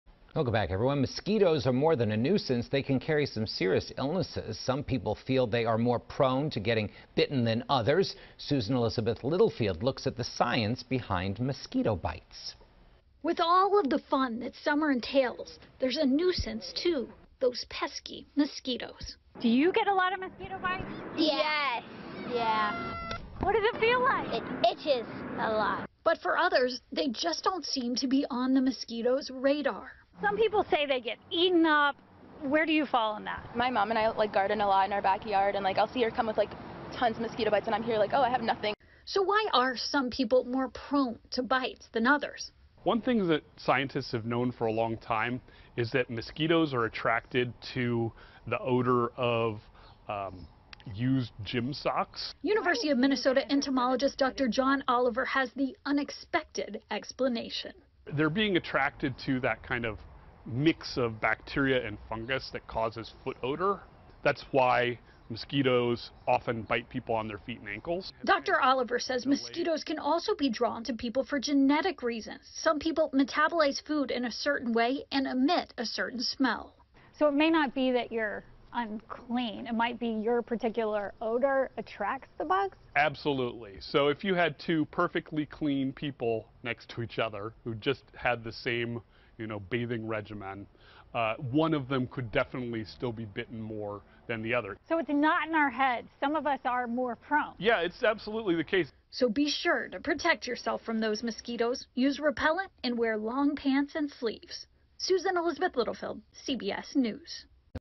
Doctor Says Mosquitos Are Attracted To Certain Bacteria Or People Due To Genetics
Susan-Elizabeth Littlefield reports.